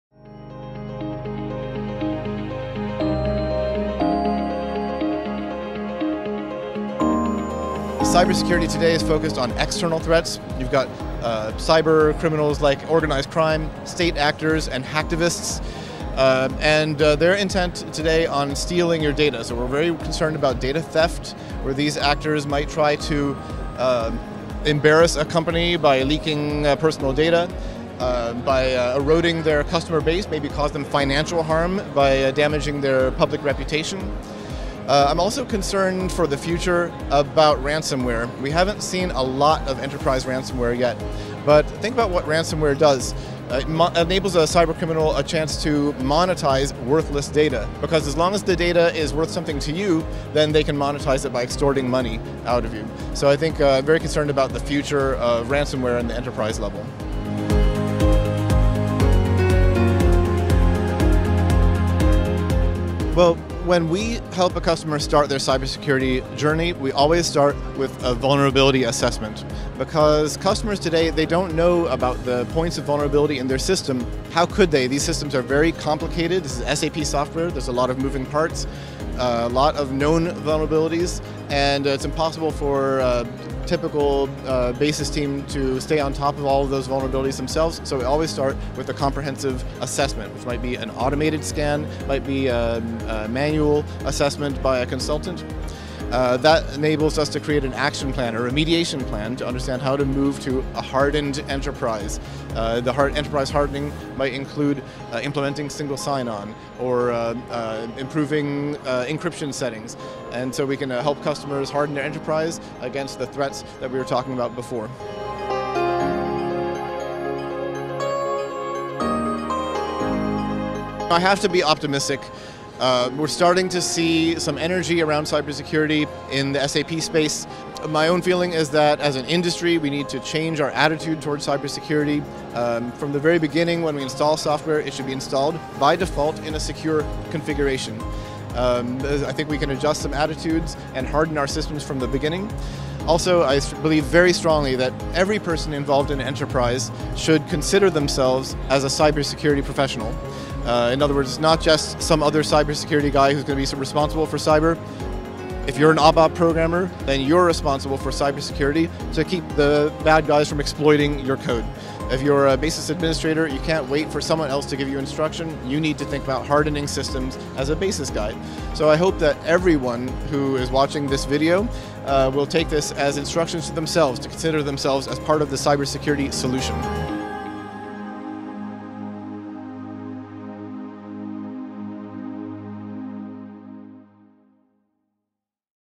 Protecting your Organization from Cybersecurity Risks
In this brief video, Jon Freisen explains the fundamentals of protecting your organization from cybersecurity risks.